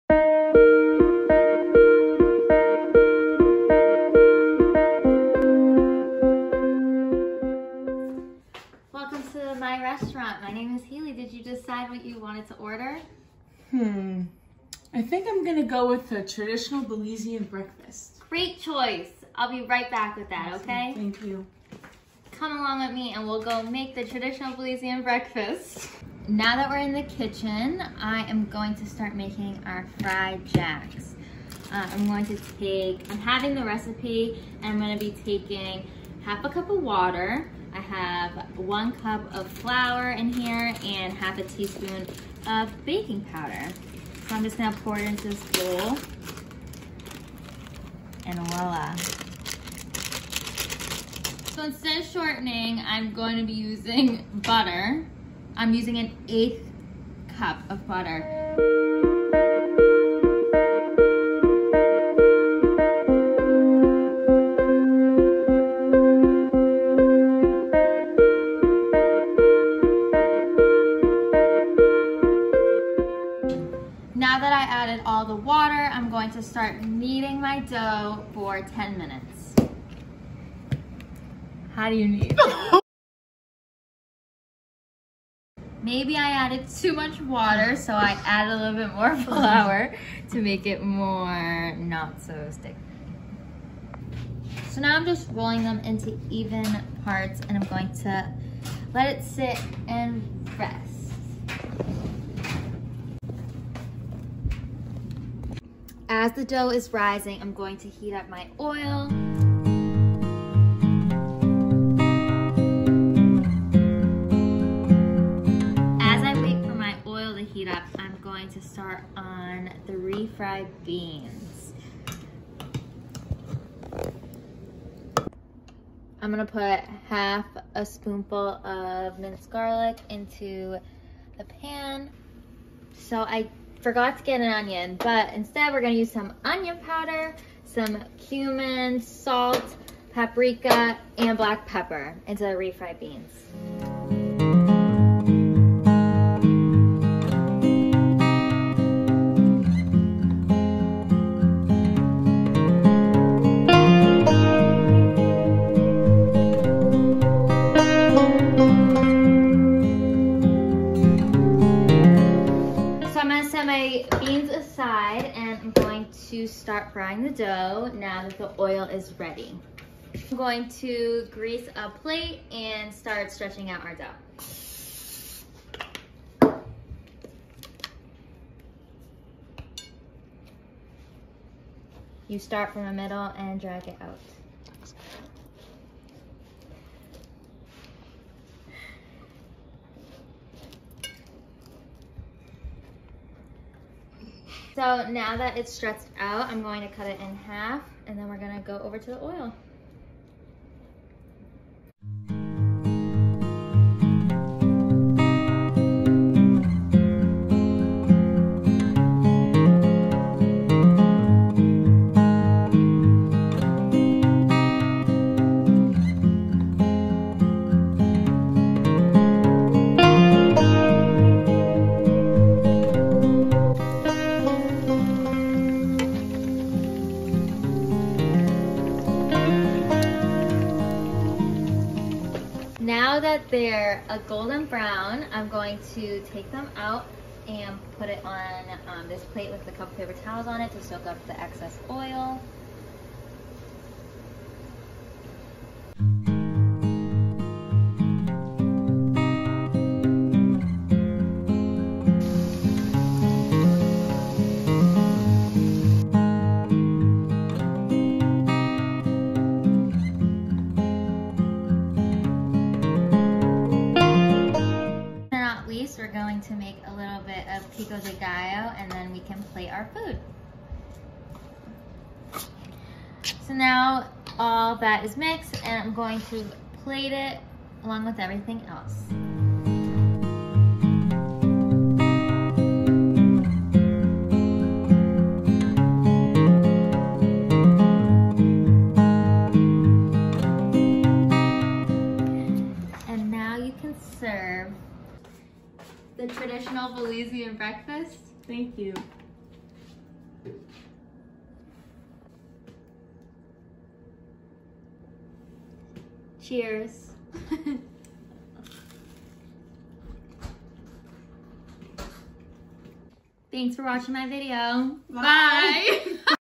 Traditional Belizean Breakfast
Traditional Belizean Breakfast for my VetMed and Wildlife Conservation in Belize class